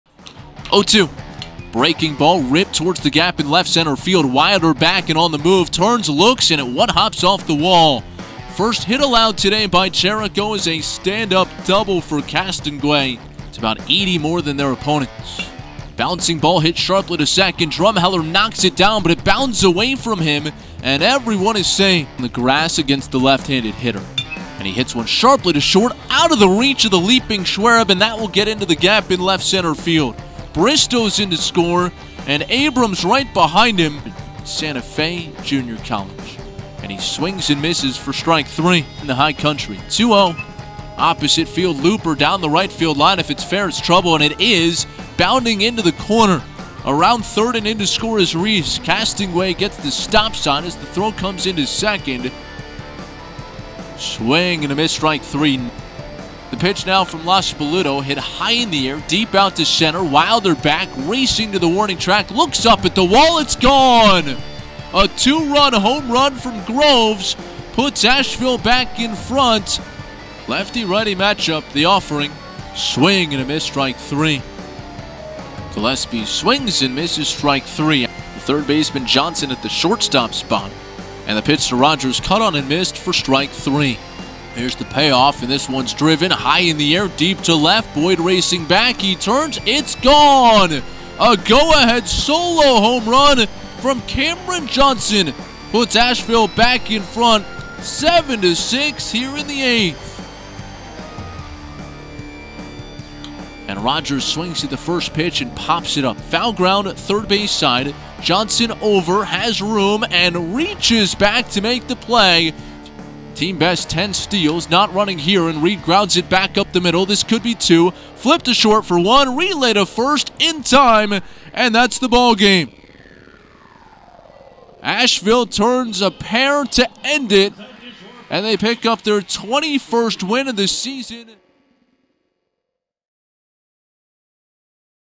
Highlights || Baseball || UNC Asheville at App State
UNC Asheville Bulldogs at App State Mountaineers Baseball Highlights (4/18/2023)